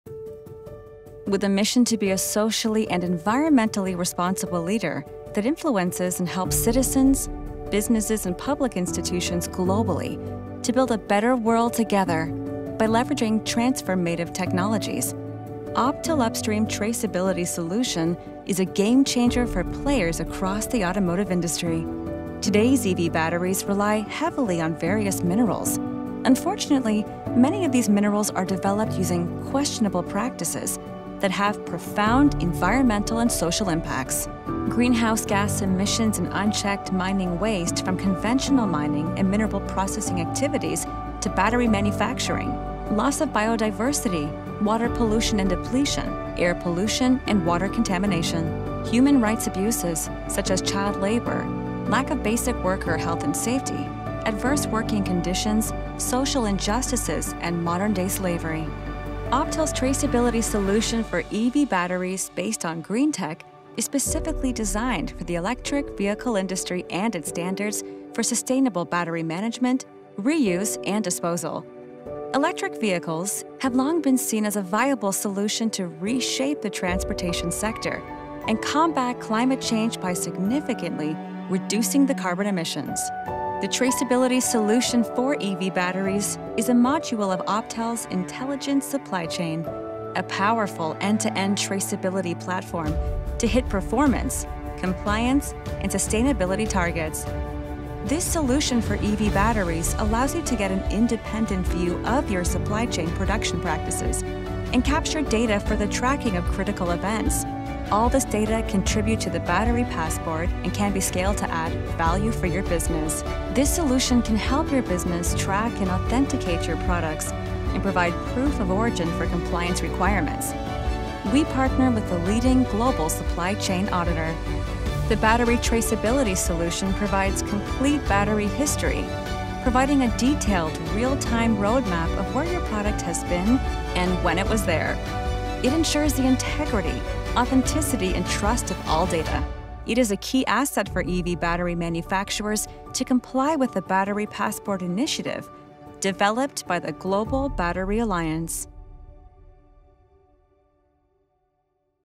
OPTEL’s Traceability Solution for Electric Vehicle Batteries
Are you hitting your ESG targets with your batteries? OPTEL’s Traceability Solution for EV Batteries, based on green tech, is specifically designed for the electric-vehicle industry and its standards for sustainable battery management, re-use, and disposal.

With ESG transparency increasingly required throughout the entire EV battery supply chain, many stakeholders are having a hard time determining which traceability solution can truly provide the real-time and in-depth information they need to prove compliance.

With the battery passport coming into effect very soon, manufacturers throughout the EV battery value chain will have to provide certifications, whether they are at the material, cell, module, battery, integration or recycling level